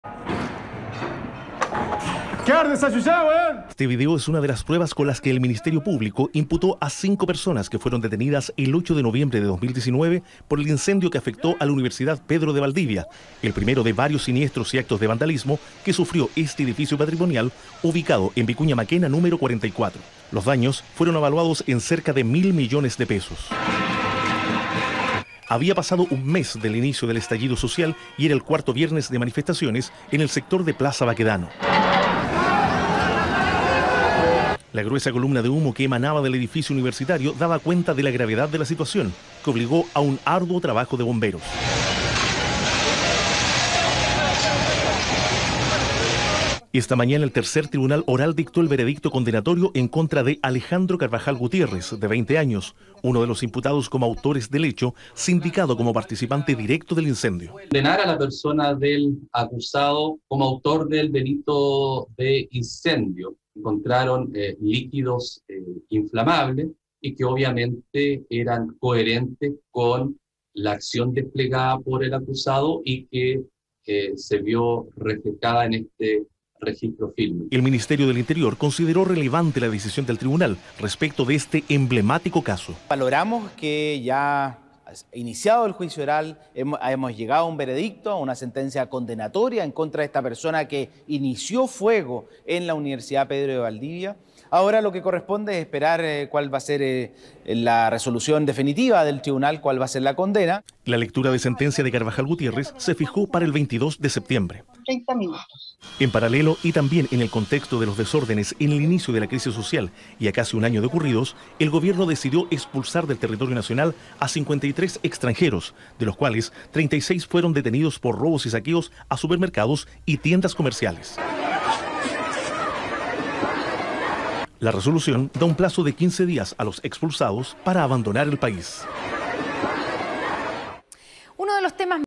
Declaran culpable a detenido por incendio de Universidad Pedro de Valdivia en noviembre
Este martes la justicia dictó veredicto en contra de unos de los acusados por el incendio del edificio patrimonial donde funcionaba la Universidad Pedro de Valdivia.